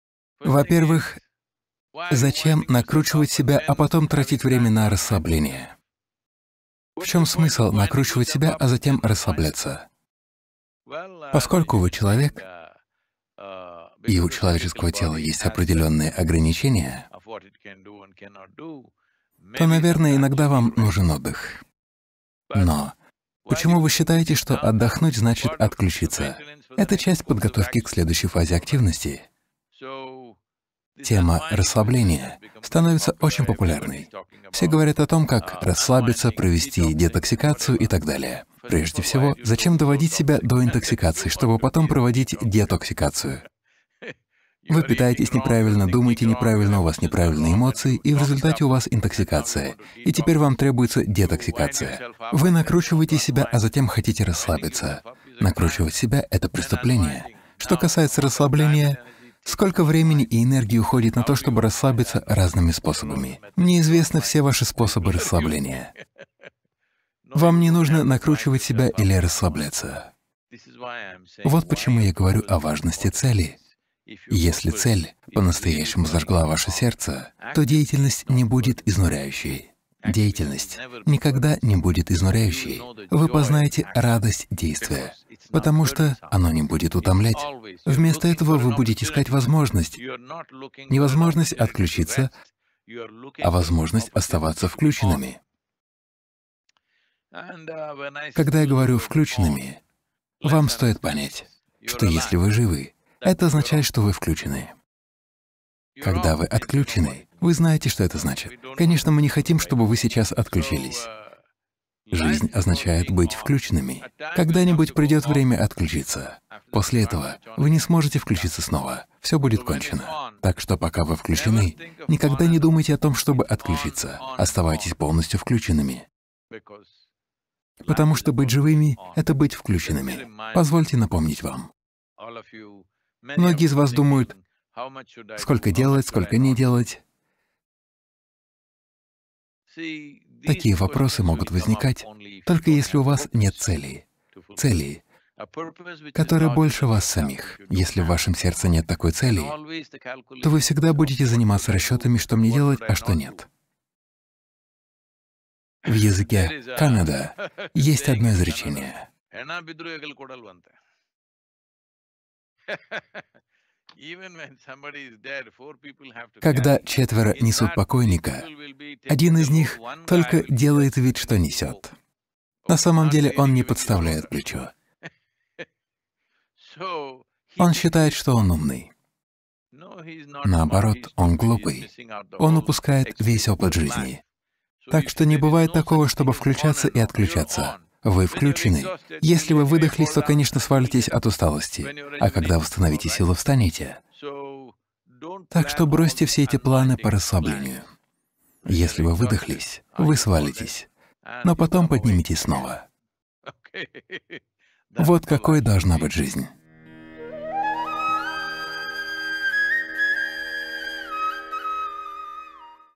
Познайте радость работать!
Ваша профессия изнуряет вас и вызывает сопротивление? Садхгуру рассказывает о том, как работать в удовольствие. Смотрите новое видео — и желание творить будет просыпаться вместе с вами.